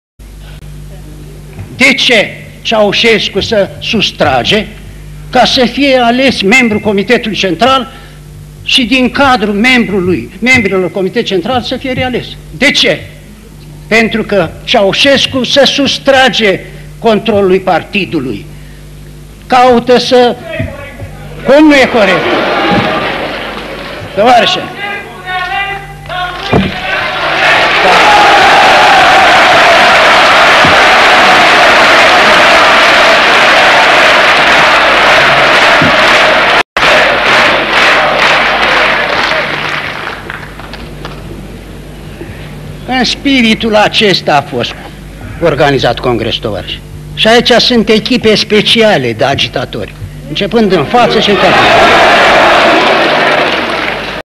Singurul om care l-a atacat pe Ceausescu. Apoi, nimeni n-a mai auzit de Constantin Pirvulescu (1979)
Comunistii s-au confruntat cu un scandal pe 23 noiembrie 1979, la Congresul al XII-lea al PCR de la Sala Palatului. Atunci, Constantin Pirvulescu, membru fondator al PCR a cerut să ia cuvântul în plenul reuniunii. Veteranul partidului a trecut peste limitele impuse de organizatorii forului de conducere, care îi cunoşteau anumite idei, în divergenţă cu linia politică generală impusă de Nicolae Ceauşescu.

Pentru a evita un scandal transmis în direct la radio si televiziune, Nicolae Ceauşescu i-a permis lui Constantin Pirvulescu sa ia cuvantul, iar acesta l-a criticat dur pe Ceausescu si realegerea lui ca secretar general al Partidului Comunist Român.

TVR nu a mai dat niciun prim plan cu Pirvulescu din momentul in care a inceput atacul la Ceausescu